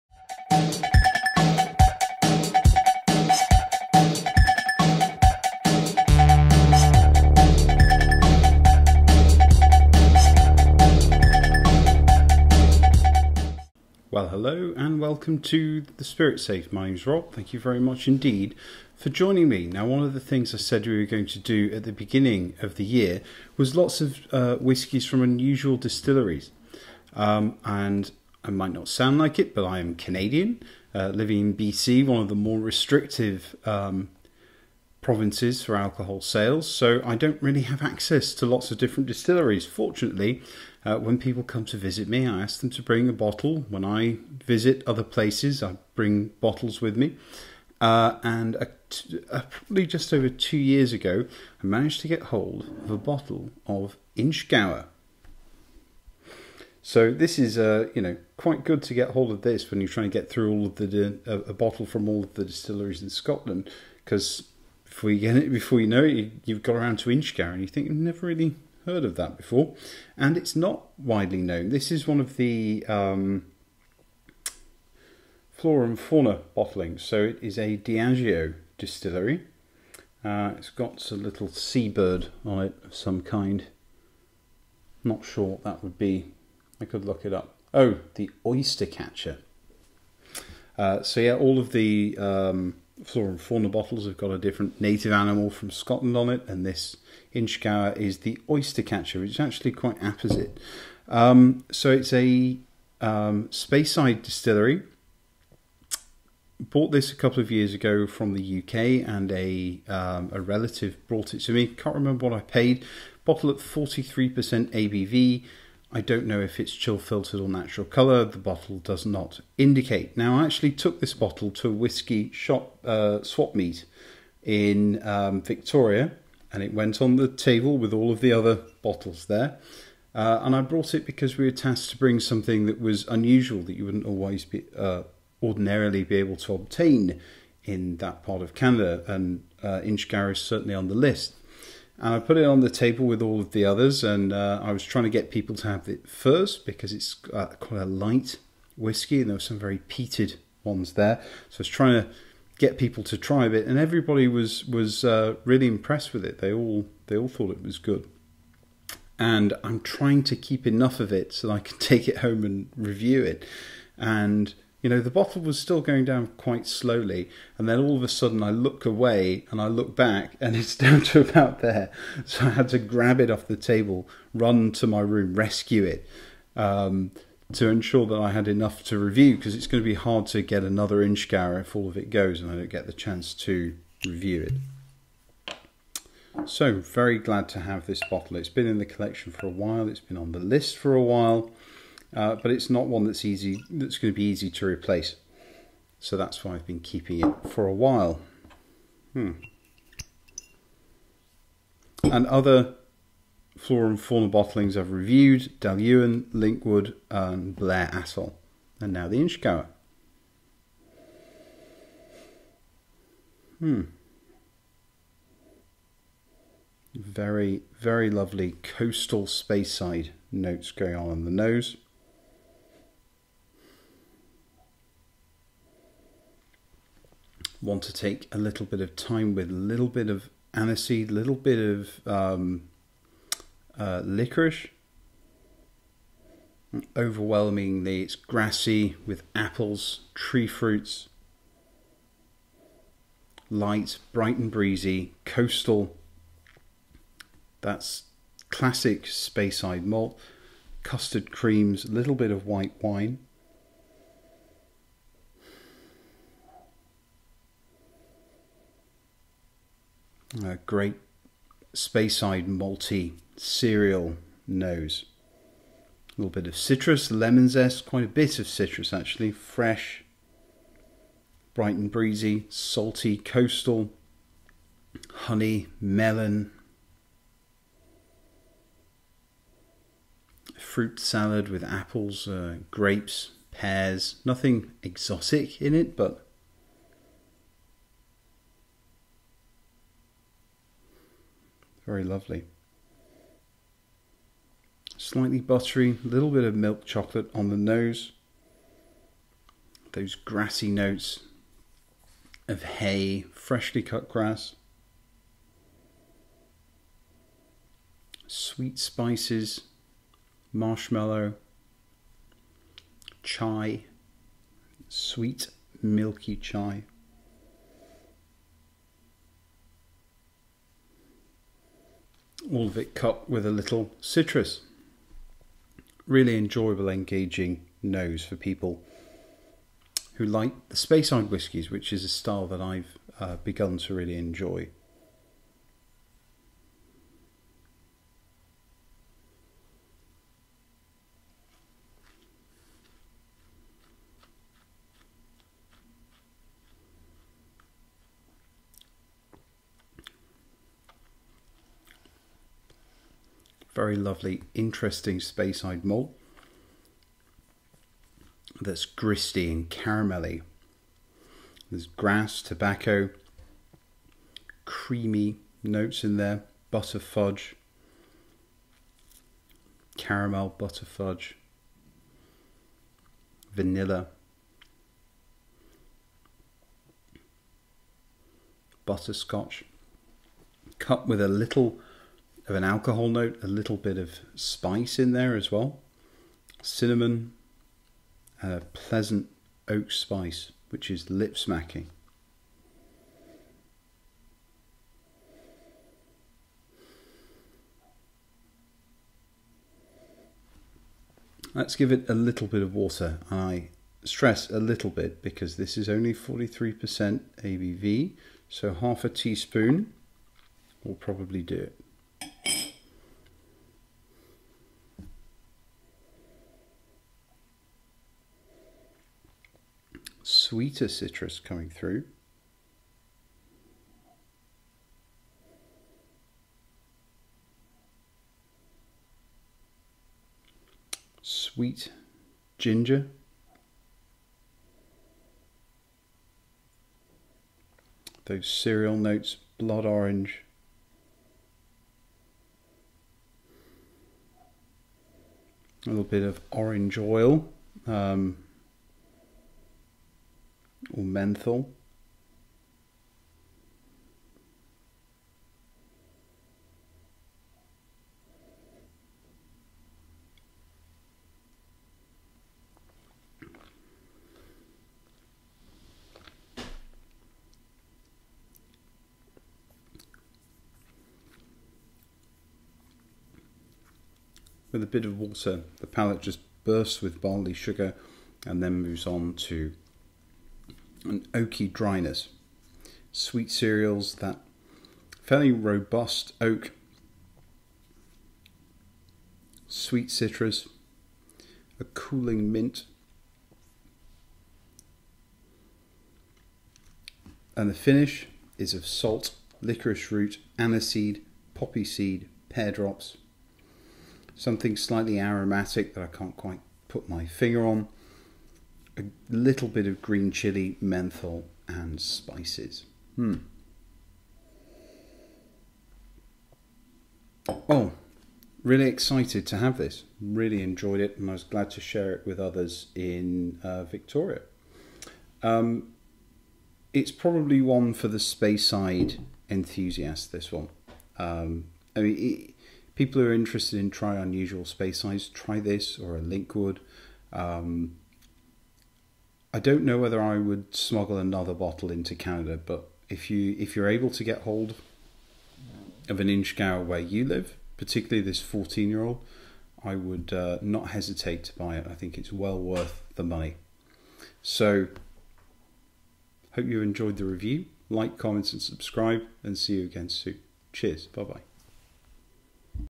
Inchgower 14 Flora & Fauna
More unusual Speyside malts! This now retails at about 48 GBP in the UK.  Well worth the buy.

Forgot to mention the herbal honey note on the finish, which is delicious.